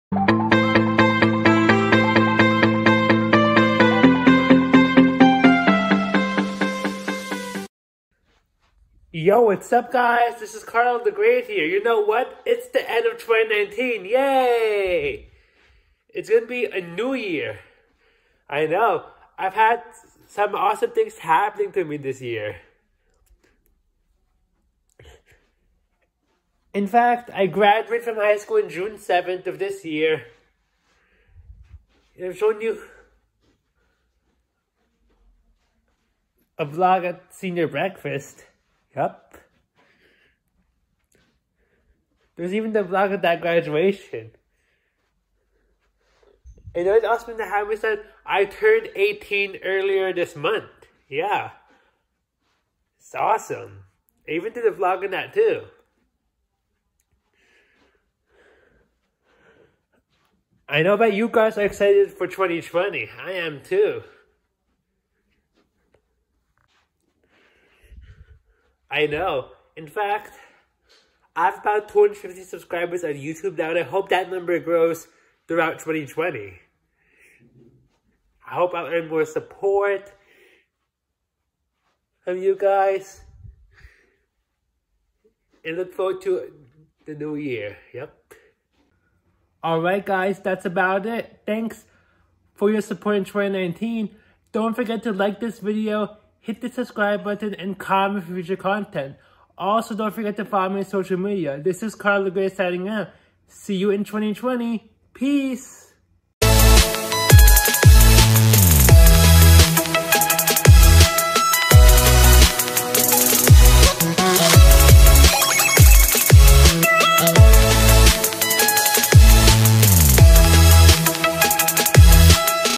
Looking Forward to 2020
It's the end of 2019, and I remember some of the awesome things that happened to me this year. At the same time, I look forward to seeing what happens in 2020.

Last video in 2019. See you in 2020!

See ya in the next video! Peace!